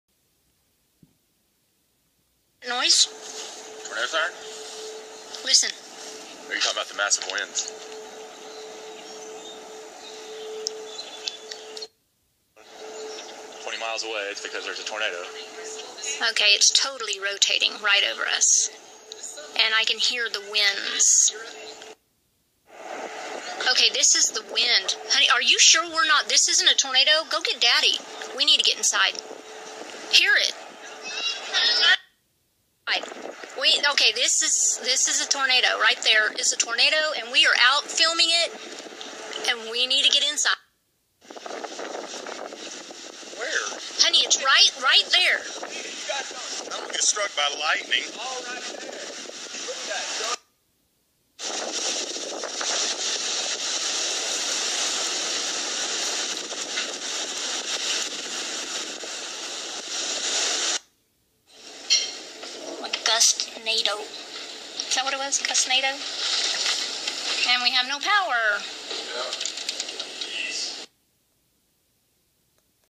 March 25, 2015. Tornado forming in Oklahoma City/Moore area.
March 25, 2015. Tornado forming at approximately 119th and S May Ave. in Oklahoma City/Moore area. It is a bit hard to see because it has not picked up debris but it is still visible.  Video credit to CGK (My sister who you hear in this series of Snap Chat video clips is speaking with her son. She did not save the clips to her gallery, so hence the video tape of the phone screen before they were lost for good. I think sometimes when you are watching the clouds you kind of can't believe what you are seeing and you keep watching to see if what you are seeing is actually what you think you are. It is mesmerizing. They lost their home to the May 3rd tornado. In fact I am completely shocked that she even taped this!) Thankful everyone was OK.